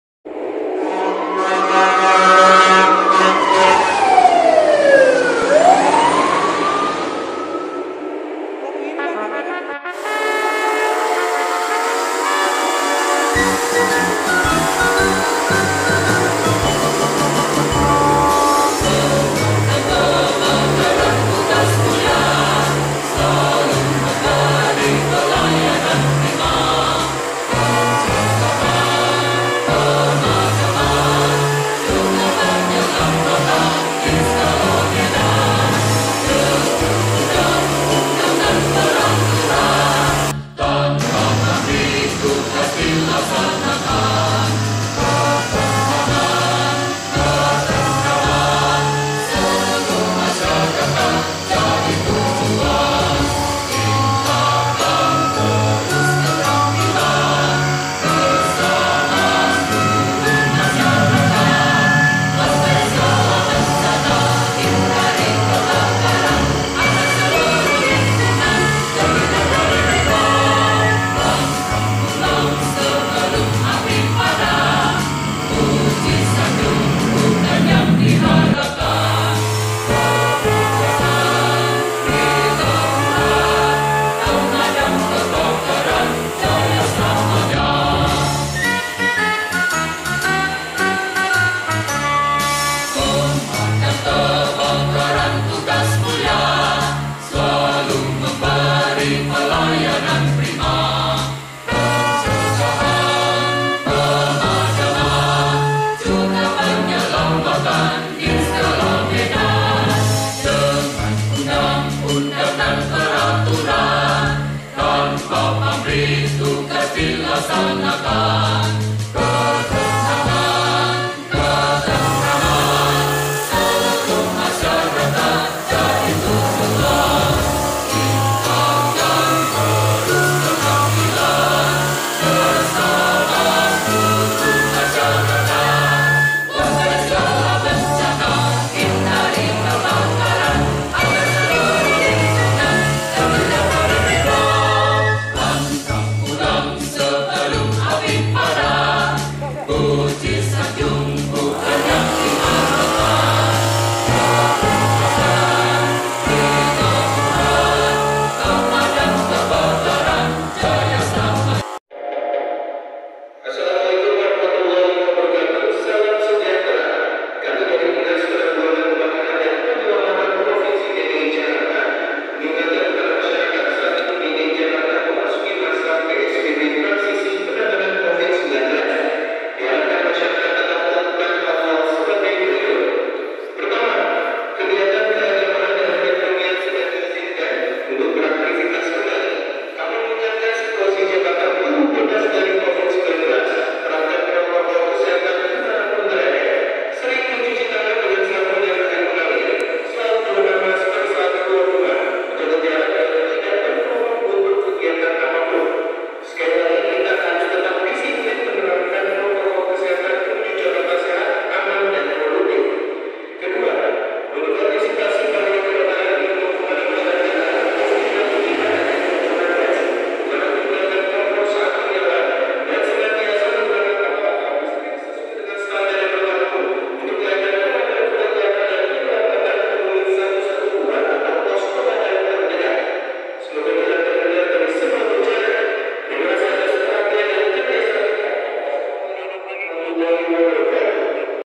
Penyemprotan Cairan Desinfektan RT.013 RW.02
Giat monitor penyemprotan cairan desinfektan bersama damkar, fkdm dan ketua RT di jl.asem 2, dan jl.mawar RT.13/02.
L.2624